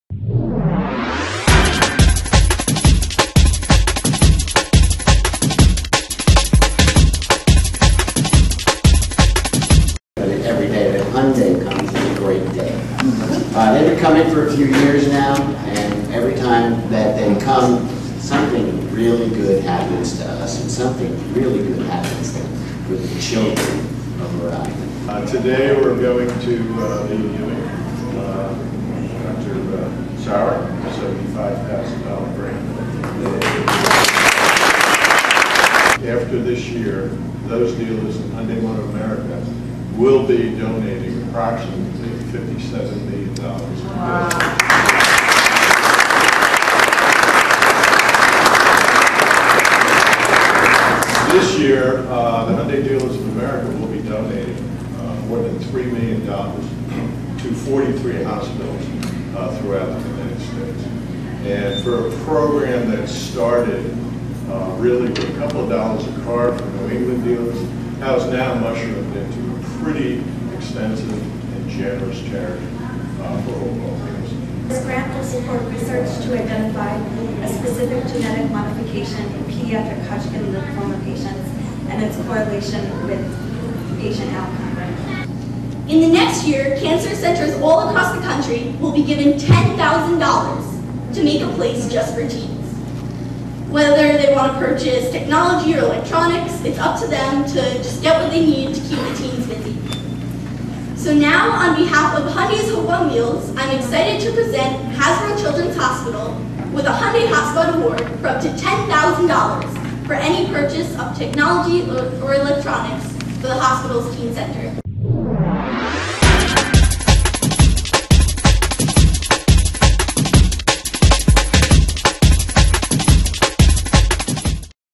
Hyundai Hope on Wheels helps Hasbro Children
CW News Now is there at Hasbro Children's Hospital as the Hyundai Hope on Wheels program gives its latest charity donation grant to Hasbro. Here's the full release: Hasbro Children's Hospital physician receives 5,000 Hyundai Scholar Grant to support pediatric cancer researchHospital has received 90,000 in support to date to better the quality of life for children with cancerProvidence, R.I. -- On Wednesday, July 18, Hyundai Hope On Wheels and Providence-area Hyundai dealers awarded Hasbro Children's Hospital a 5,000 Hyundai Scholar Grant to Nadine Sauer, M.D. The grant supports Dr. Sauer's research work focusing on whether an individual's genetic makeup can predict their vulnerability to chemotherapy-related toxicities. The goal is to modify treatments to minimize side effects. Hasbro Children's Hospital is one of 43 recipients of the Hope On Wheels 2012 Hyundai Scholar Grant, which supports principal investigators—known as Hyundai Scholars—in their efforts to improve the lives of children battling pediatric cancer. "Dr. Sauer's research is a vital piece of the fight to find a cure for childhood cancer and to better the quality of life for children battling this disease," said Cindy Schwartz, M.D., director of hematology/oncology at Hasbro Children's Hospital. "Our institution has received 90,000 from Hope On Wheels since 2010, and Hyundai continues to be an important supporter of our pediatric cancer research program.  Their investment in our work has made a tremendous impact."The 5,000 Hyundai Scholar Grant to Dr. Sauer was officially presented during a Hope On Wheels handprint ceremony at Hasbro Children's Hospital, where children affected by cancer dipped their hands in colorful paint and placed their handprints on a new Hyundai Tucson, the official vehicle of the program. Every handprint ceremony celebrates the courageous lives of the children faced with cancer and brings Hope On Wheels' community of supporters together.   In addition to the 5,000 Hyundai Scholar Grant, Hasbro Children's Hospital is also being awarded with an additional up to 0,000 toward the purchase of personal technology and electronics as part of the Hyundai Hot Spot Award, which is designed to bolster hospital activities for teen patients."We see Hope On Wheels as a critical part of our mission at Hyundai," said John Krafcik, president and chief executive officer of Hyundai Motor America. "Pediatric cancer takes far too many young lives and impacts far too many families. We are proud to work with our Hyundai dealers to fund research to end this disease.  We congratulate each doctor and institution receiving our grants, and commend them for the important work they are doing."